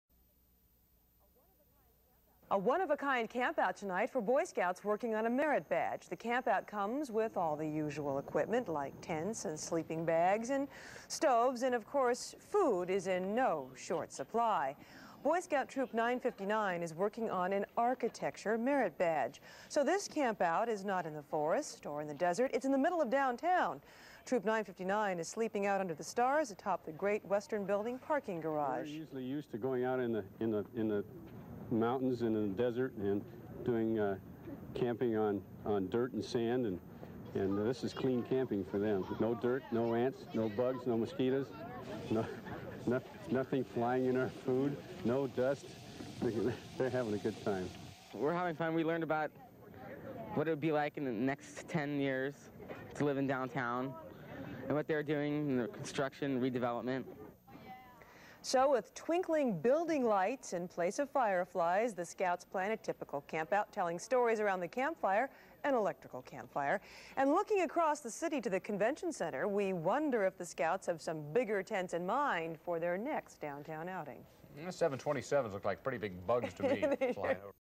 San Diego BSA Troop 959 Campout 1990
Boy Scout Troop 959 chartered by San Carlos United Methodist Church camping overnight on roof of downtown San Diego bank building and studying for architecture merit badge.  Story from KGTV weekend newscast around 1990